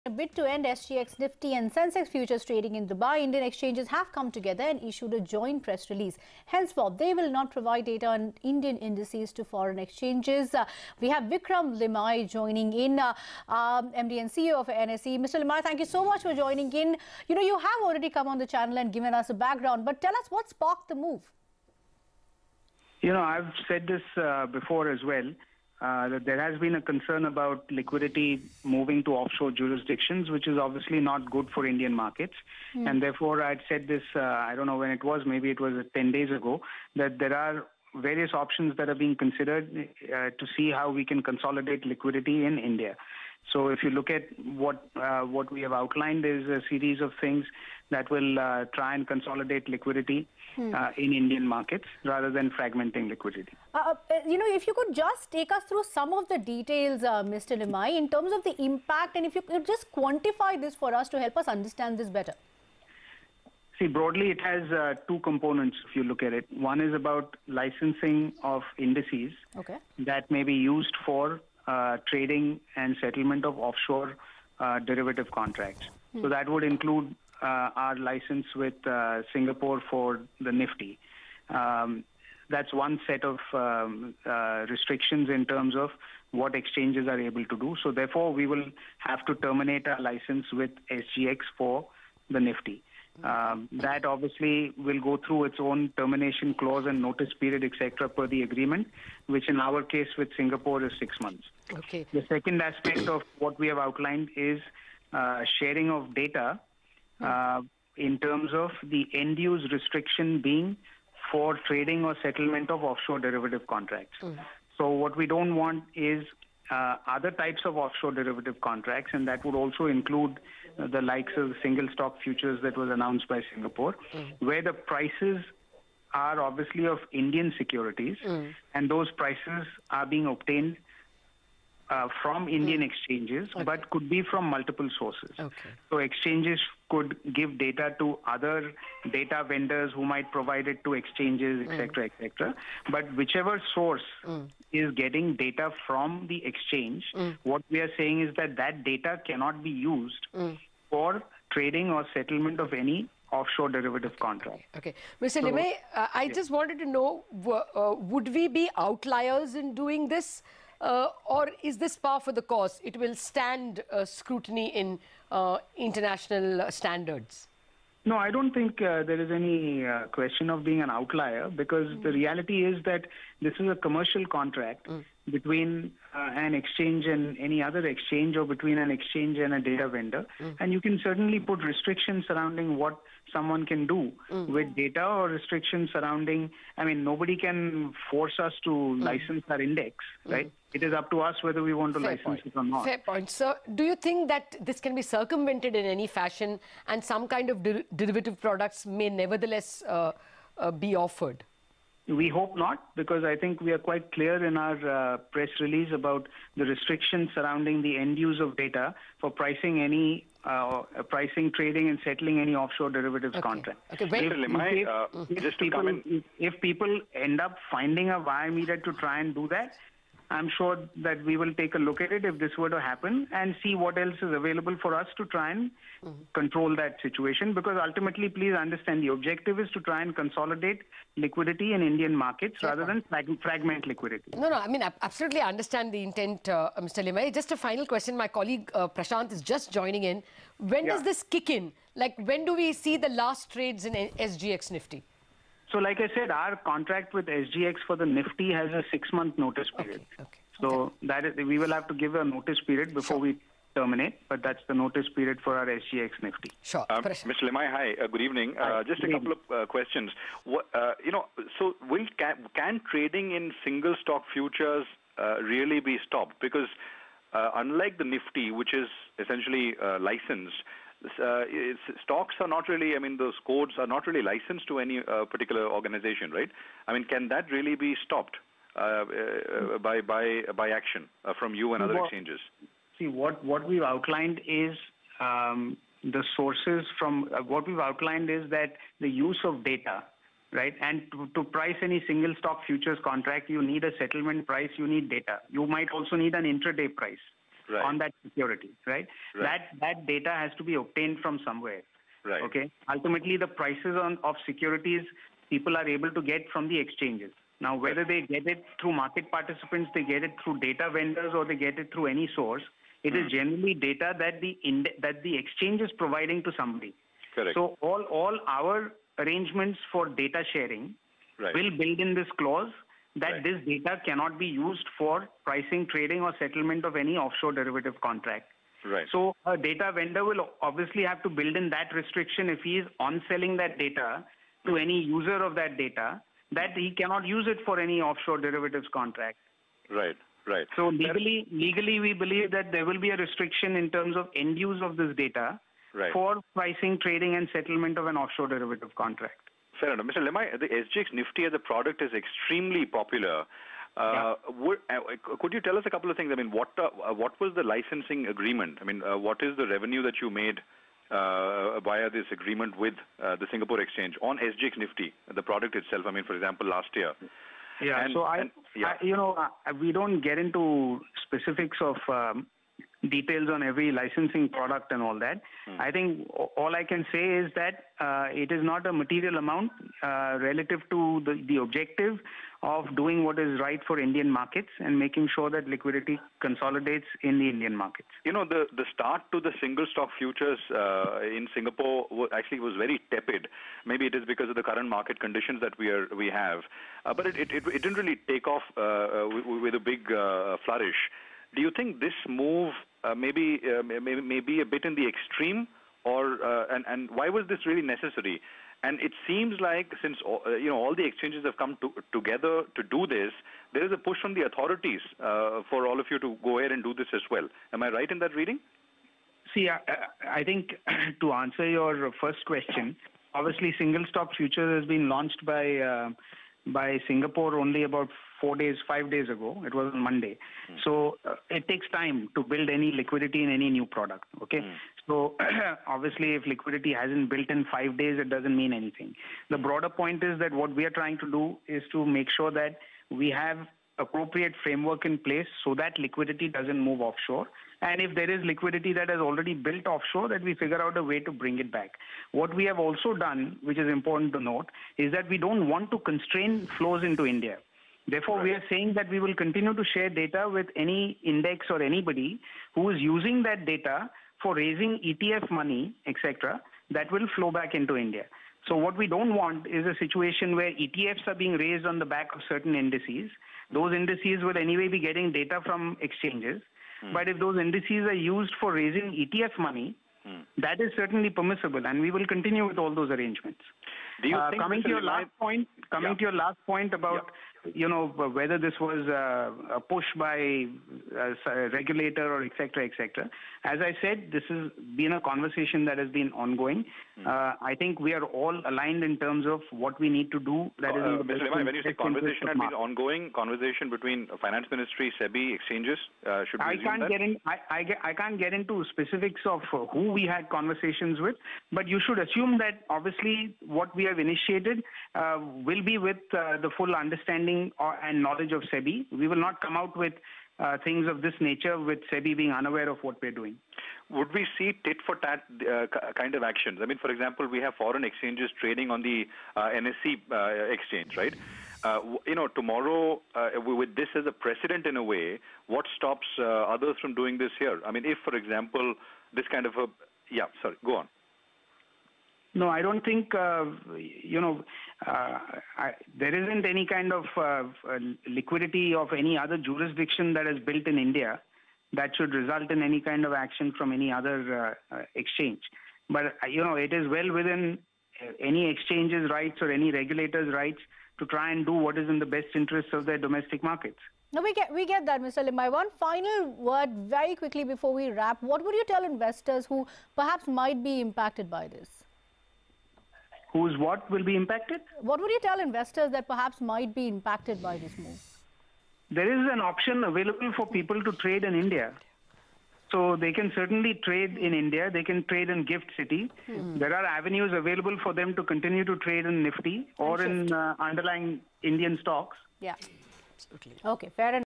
NSE, BSE Issue Joint Press Release To End SGX Nifty & Sensex Futures | CNBC TV18
Indian exchanges have issued a joint statement saying that they won't provide data on Indian indices to foreign exchanges, henceforth. Here's what Vikram Limaye, MD & CEO of NSE had to say.